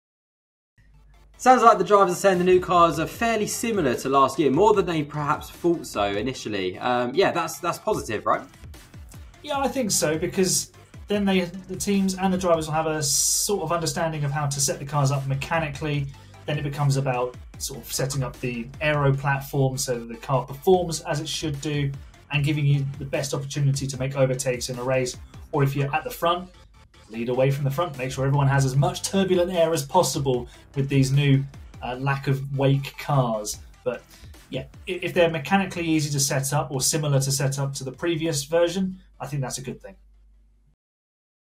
F2 is BACK! New Car or Same Car?
2024 Formula 2 is back and we start the season looking at the new Dallara Spec F2 car. What do you think?

🔵 InsideF2 Social Media 🌐

Image Credits: 📸 Formula Motorsport Limited